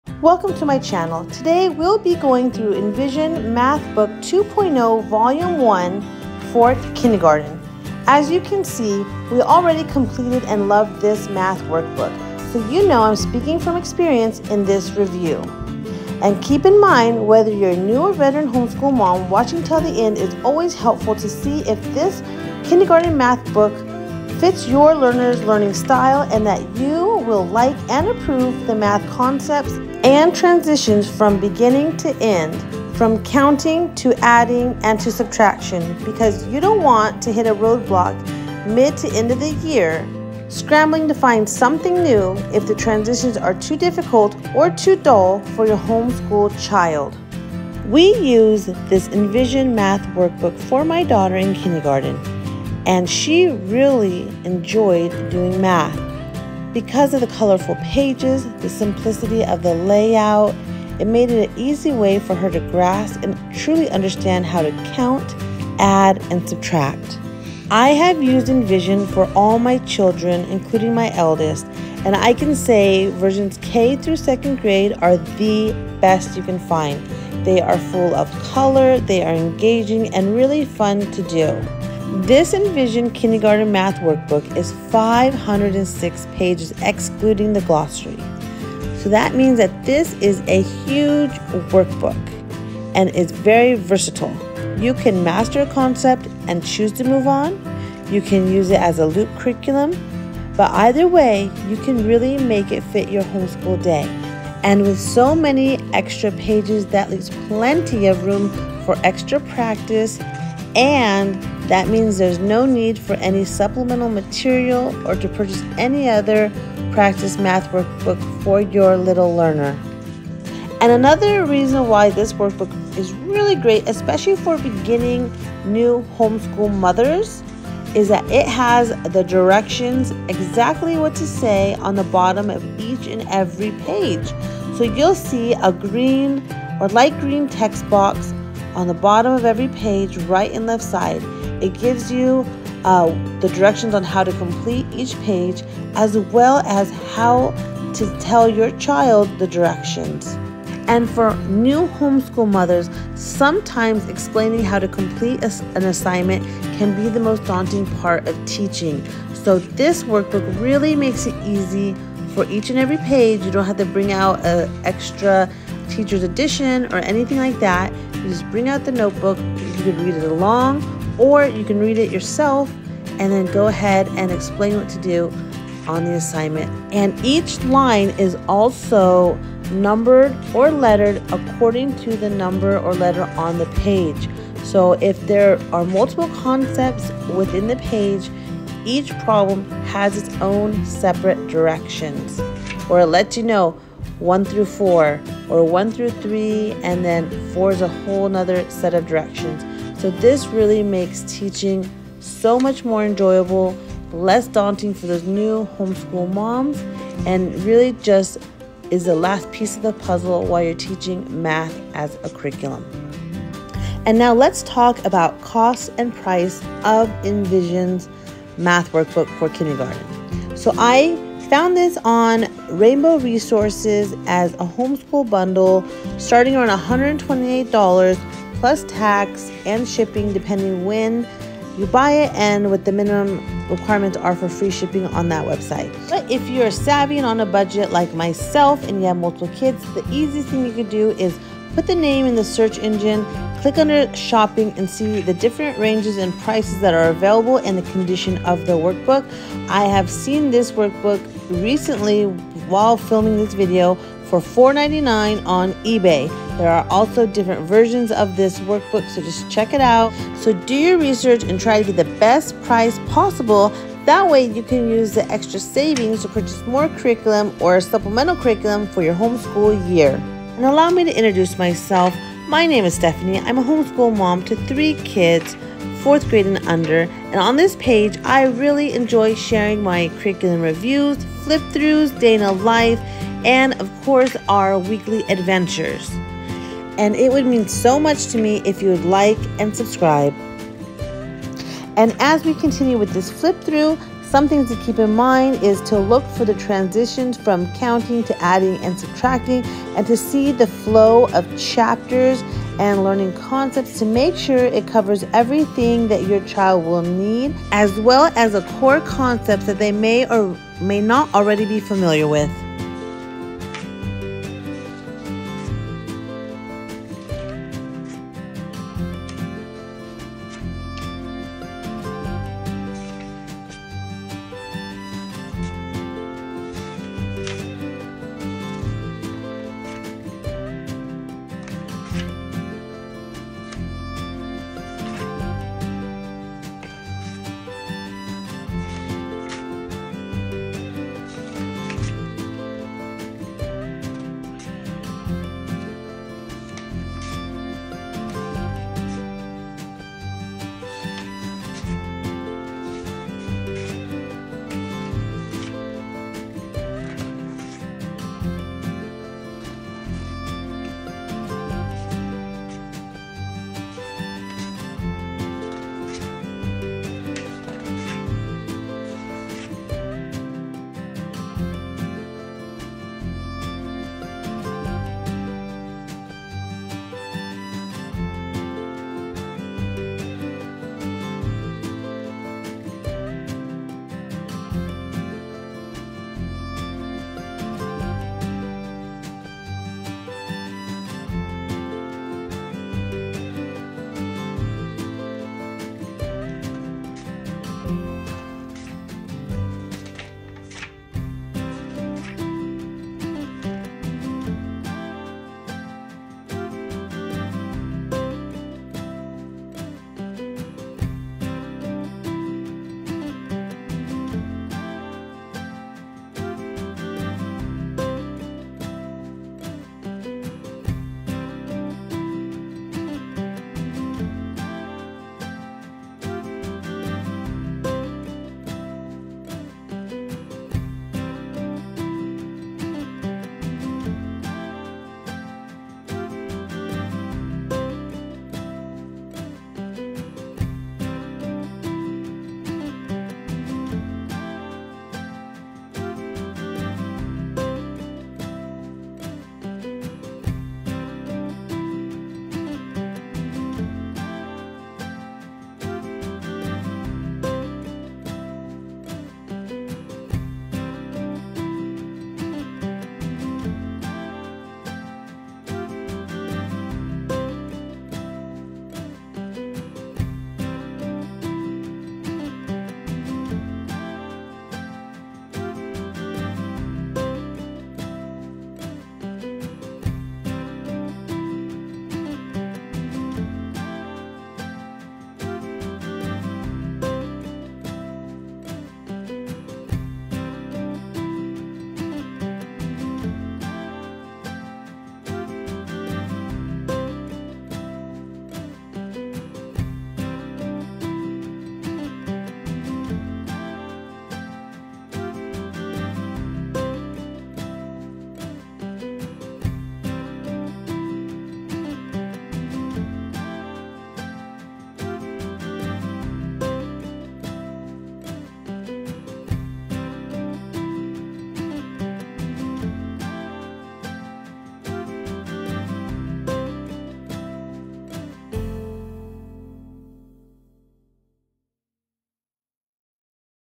Envision Math 2.0 Volume 1 Flip Through and Review
Envision Math workbook is perfect for a Nee or Veteran Homeschool mom. Check out this honest flip through and review to see why!

~ Stephanie